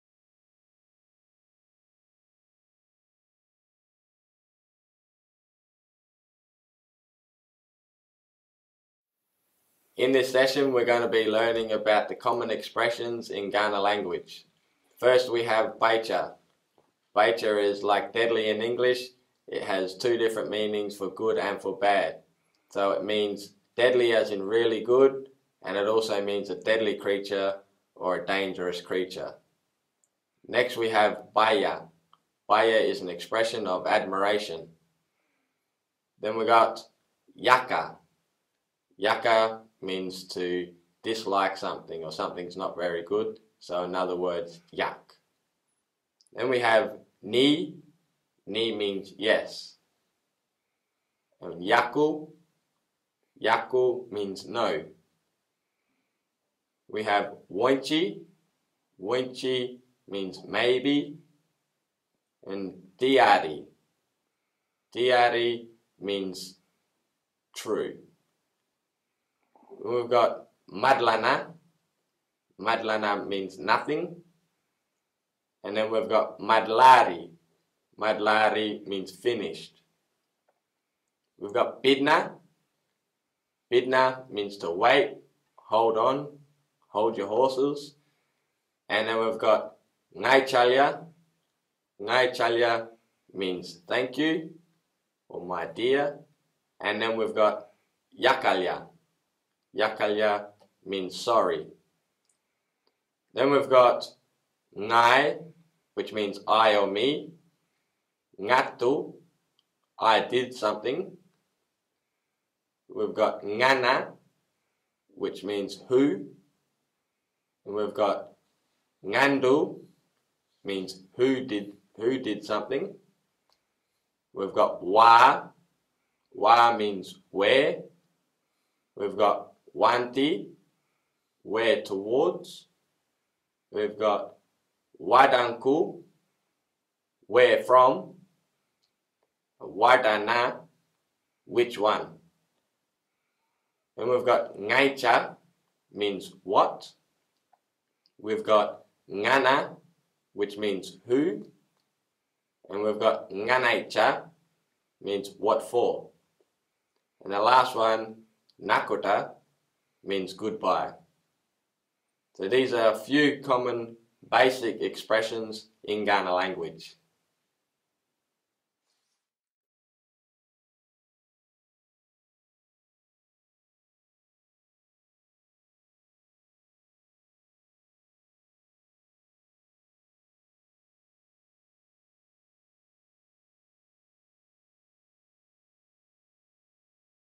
lesson 6, Common Expressions
In this series Jack Buckskin outlines some basic words and phrases of the Kaurna language.  The Kaurna are the original inhabitants of the Adelaide plains area in South Australia.  For more information on the Kaurna language visit the KWP website - 'adelaide.edu.au/kwp'